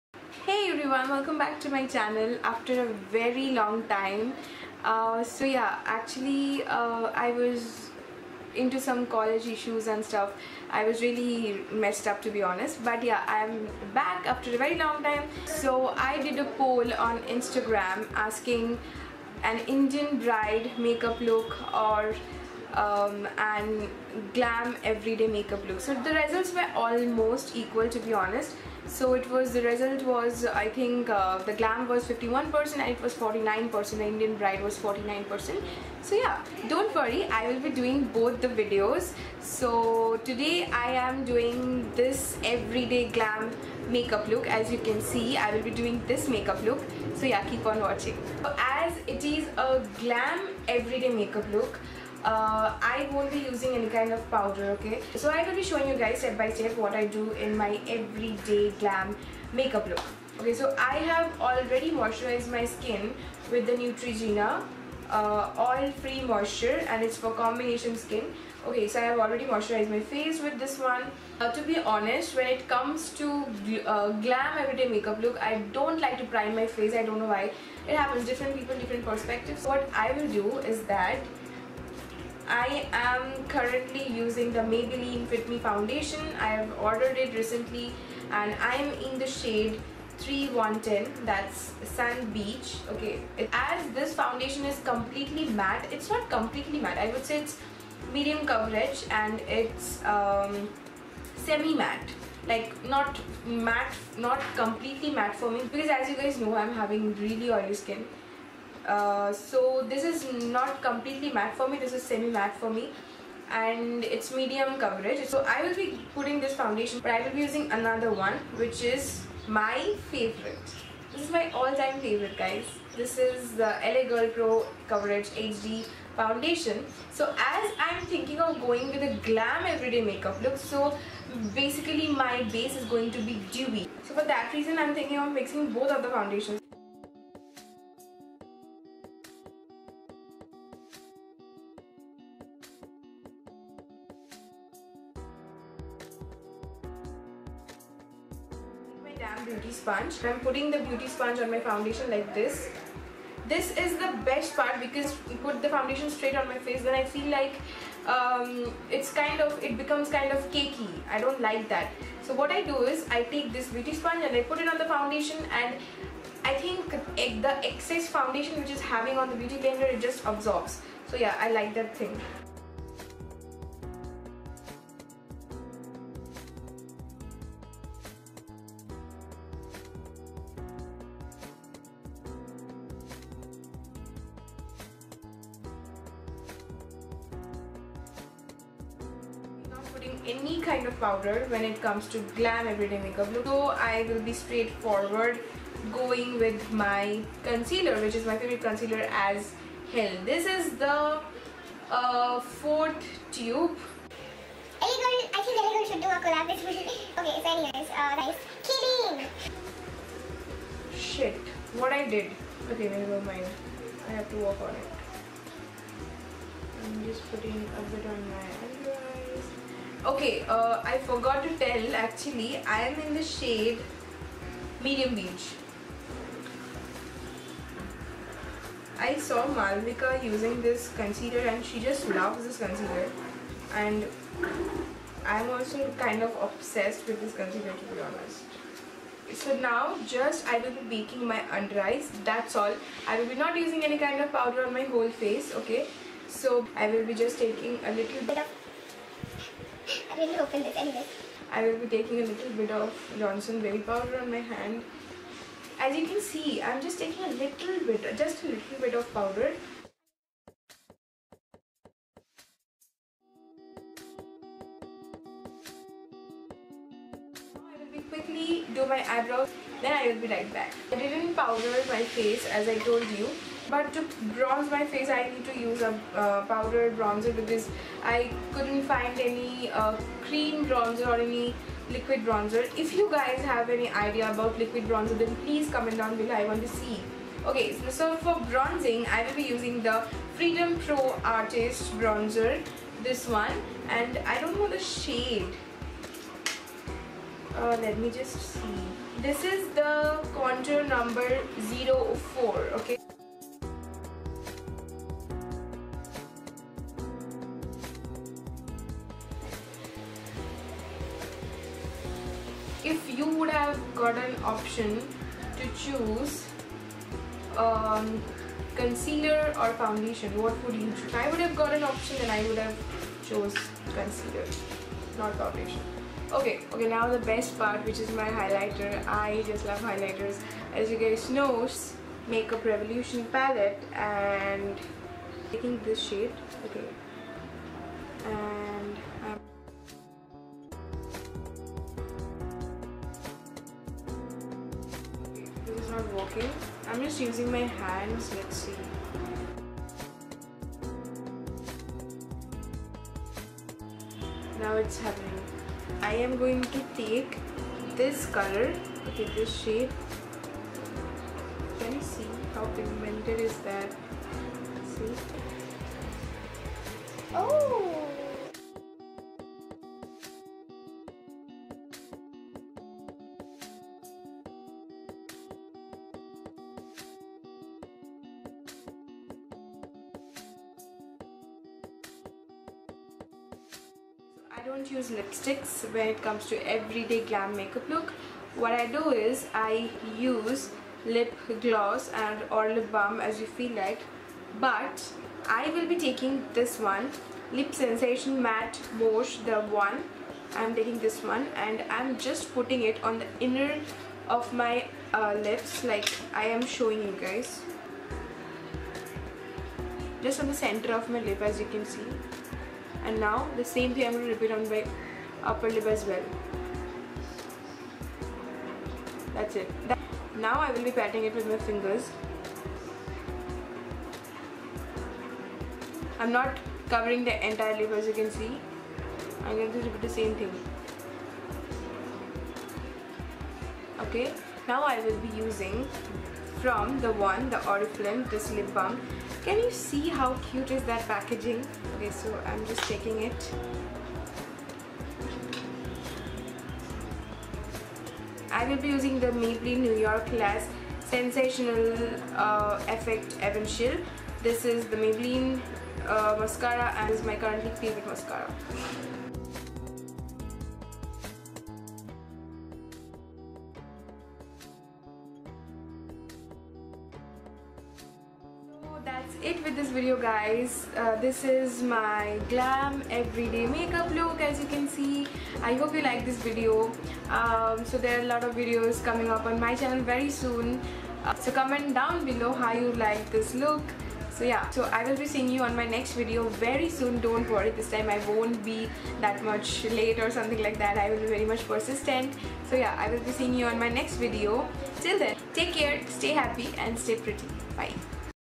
GLAM EVERYDAY MAKEUP LOOK.
HELLO everyone.
i am back again with another makeup video as you guys requested me to do this makeup look.

stay happy an pretty.